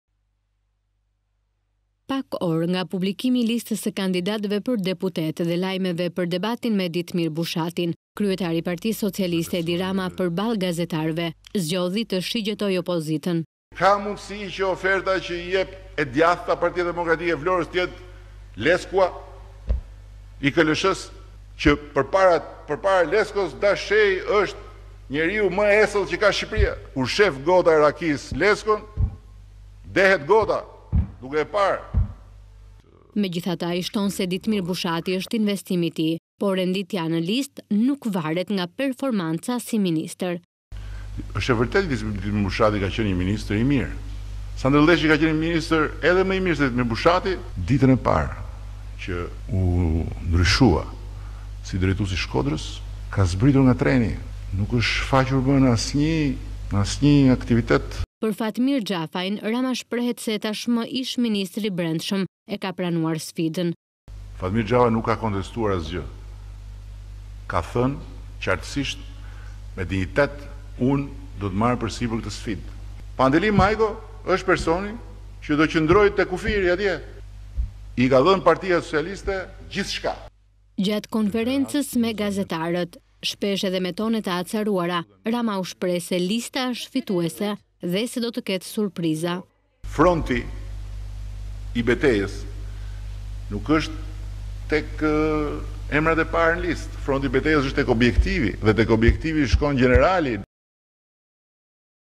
Rama akuza Ditmir Bushatit: Harroi socialistët
Nga selia e PS-së, kryeministri  Edi Rama është shprehur se partia e tij ka një skuadër që afirmon  njerëz  me eksperiencë e me energji të re.

Edi Rama ka komentuar edhe vendosjen poshtë listës së sigurt të emrave që kane kontribuar në PS, duke filluar nga Fatmir Xhafaj dhe Pandeli Majko.

Rama: Nuk kam ndryshuar mendje për Fatmit Xhafajn. Ai nuk ka bërë naze për vendin. Ka marrë vendin ku është dhe do të përballojë sfidën që ka. Leximi i një skuadër bëhet kështu. Se lojtarët nuk shihen nga gjendja e stadiumit. Në Tiranë duke qenë se nuk kemi drejtues që të jetë deputet, Pandeli Majko është personi që do të qëndrojë te kufiri. PS i ka dhënë gjithçka. Është në njësinë që është vatra më e nxehtë, hambari i grurit ndërkohë që gjithë të tjerët shkojnë për misra. Ka gjithçka që i duhet që ta çojë këtë skuadër drejt 25 prillit me gjithë atë që përfaqëson.

Kryeministri, Edi Rama, ka pasur edhe një mesazh për  kritikët e listës, me në krye Ditmir Bushatin, duke thënë se debatet me këtë të fundit janë konceptuale.

Rama: Shokët e Ditmir Bushatit sot janë tek kufiri, e jo se kanë bërë debate me mua, por ata përveç punës si ministra,  përvec mbledhjeve dhe daljeve në televizor kanë punuar tokën, kanë mbjedhë e kultivuar në terren me njerëzit përditë të korrurat e 25 prillit, ndërsa Ditmir Bushati ditën e parë që u ndryshua si drejtues i Shkodrës ka zbritur nga treni dhe nuk është shfaqur në asnjë aktivitet në ato aktivitete që nuk janë puna juaj nuk janë televizive por pjesë e pandashme e çdo njeriu që kanë një mandat në këtë parti.

Është shumë e thjeshtë. Nëse Bushati do kishte punuar tokën, do ishte njësoj si shokët e vet. Kjo nuk është personale. Kush është lart duhet të rrijë poshtë, jo në listë, duhet të rrijë poshtë çdo ditë.

Një lojtar unë e mas me sa duar njerëzish ka shtrënguar sa fshatarë ka takuar, sa biznese ka takuar për t’iu thënë si ju shkojnë punët, çfarë keni nevojë, sa qytetarë ka takuar, sa i di ai problemet e zonës ku është atje se nuk mund të jetë historia që shkojnë te njerëzit vetëm për  fushatë.Shpresoj ta kape trenin prapë. Partia të vë por nuk të mban, mban veten.